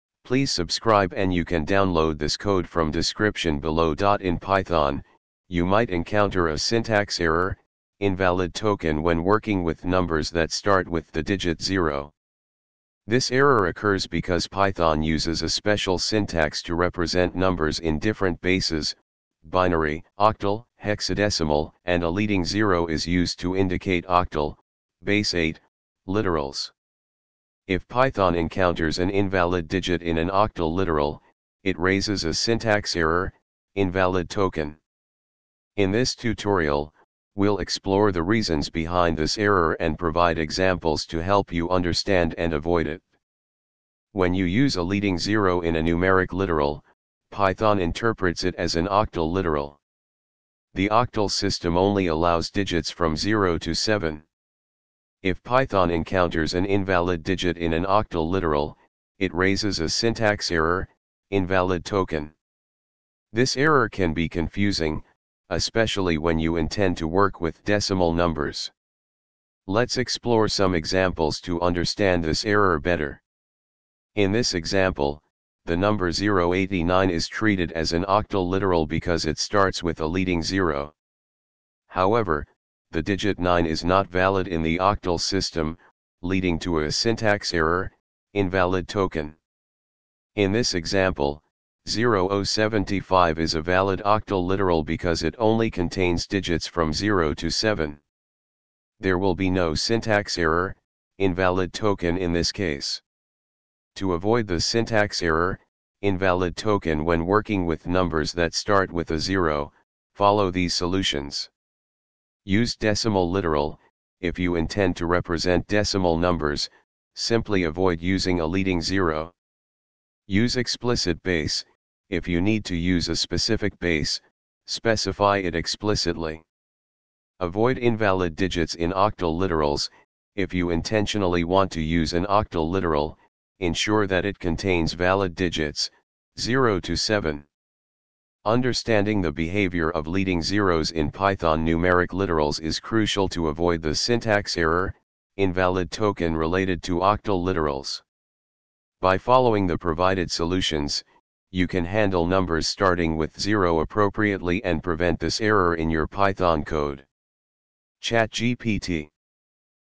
Python SyntaxError invalid token on numbers starting with 0 zeroes
In Python, you might encounter a "SyntaxError: invalid token" when working with numbers that start with the digit 0. This error occurs because Python uses a special syntax to represent numbers in different bases (binary, octal, hexadecimal), and a leading zero is used to indicate octal (base-8) literals. If Python encounters an invalid digit in an octal literal, it raises a "SyntaxError: invalid token."
In this tutorial, we'll explore the reasons behind this error and provide examples to help you understand and avoid it.
When you use a leading zero in a numeric literal, Python interprets it as an octal literal. The octal system only allows digits from 0 to 7. If Python encounters an invalid digit in an octal literal, it raises a "SyntaxError: invalid token." This error can be confusing, especially when you intend to work with decimal numbers.
Let's explore some examples to understand this error better.
In this example, the number 089 is treated as an octal literal because it starts with a leading zero. However, the digit 9 is not valid in the octal system, leading to a "SyntaxError: invalid token."
In this example, 0o75 is a valid octal literal because it only contains digits from 0 to 7. There will be no "SyntaxError: invalid token" in this case.
To avoid the "SyntaxError: invalid token" when working with numbers that start with a zero, follow these solutions:
Use Decimal Literal: If you intend to represent decimal numbers, simply avoid using a leading zero.
Use Explicit Base: If you need to use a specific base, specify it explicitly.
Avoid Invalid Digits in Octal Literals: If you intentionally want to use an octal literal, ensure that it contains valid digits (0 to 7).
Understanding the behavior of leading zeros in Python numeric literals is crucial to avoid the "SyntaxError: invalid token" related to octal literals. By following the provided solutions, you can handle numbers starting with zero appropriately and prevent this error in your Python code.
ChatGPT